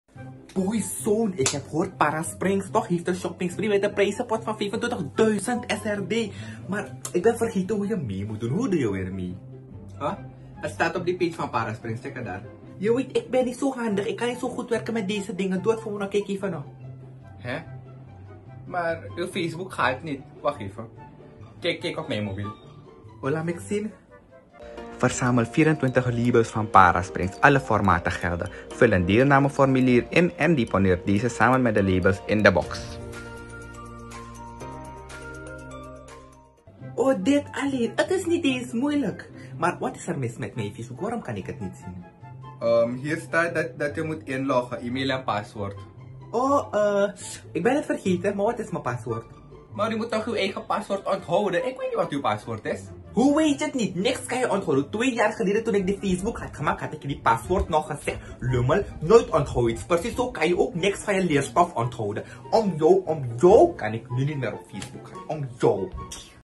Para Springs Shopping Spree
Doe mee en win❗❗❗

De prijzenpot is SRD 25.000

HOE DOE IK MEE❓❓❓❓🥳🥳

•Spaar 24 Para Springs Labels🔷️🔷️🔷️🔷️
•Vul uw deelnameformulier in✍✍✍✍🧾
•Hecht de 24 Para Springs labels samen met uw deelnameformulier🖇
•Deponeer dan in onze inleverbussen🗳🗳🗳
📍LOCATIES WAAR U UW DEELNAMEFORMULIEREN KUNT HALEN EN DEPONEREN: 👇👇👇👇
●Best Mart, Jozef Israelstraat
●Choi's Noord, Tweekinderen weg
●Choi's Zuid, Johannes Mungrastraat
●Lins Supermarkt, Gemenelandsweg
●G-Sale, Johannes Mungrastraat
●Choi's Lelydorp, Indira Gandhiweg
●Zheng Warenhuis, Indira Gandhiweg